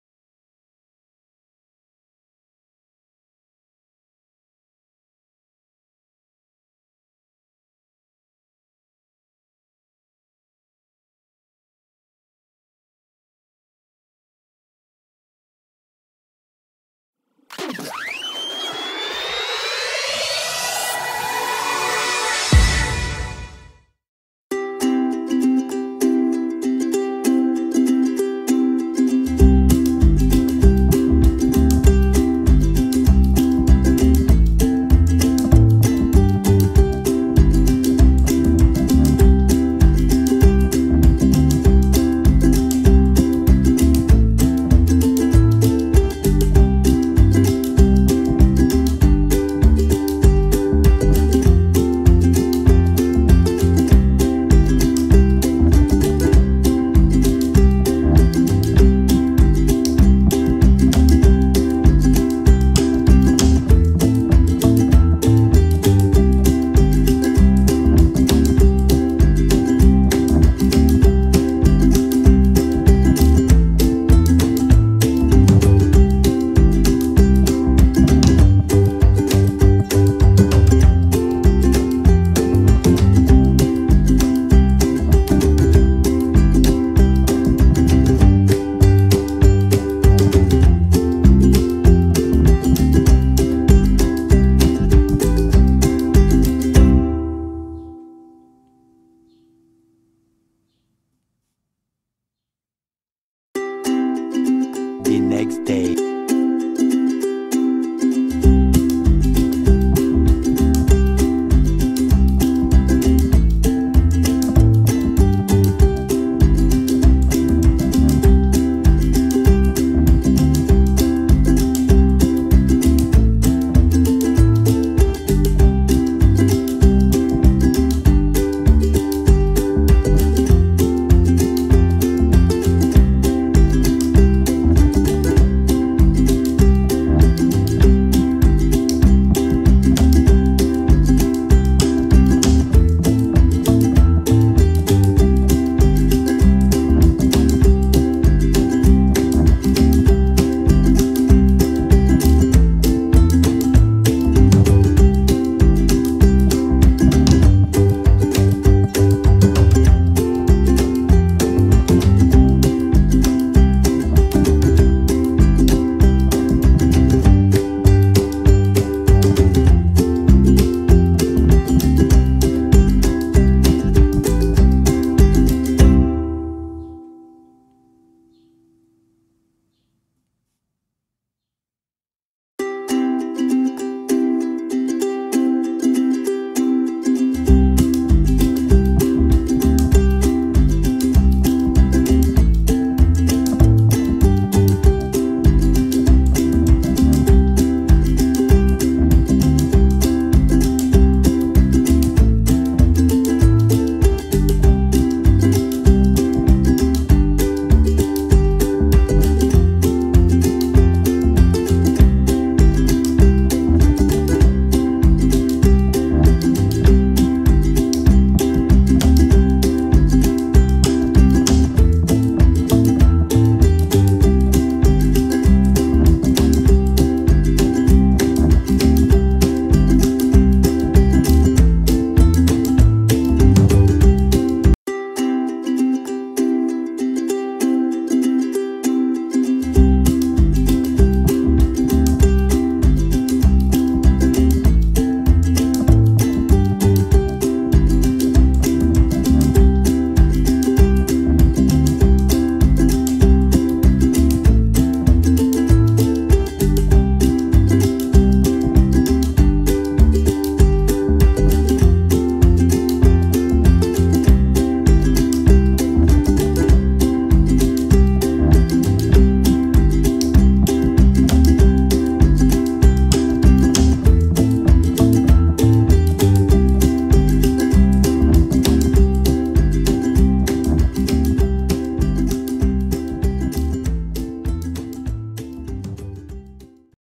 Tutorial on how to plant cat grass.
This video is about planting cat grass and its benefits.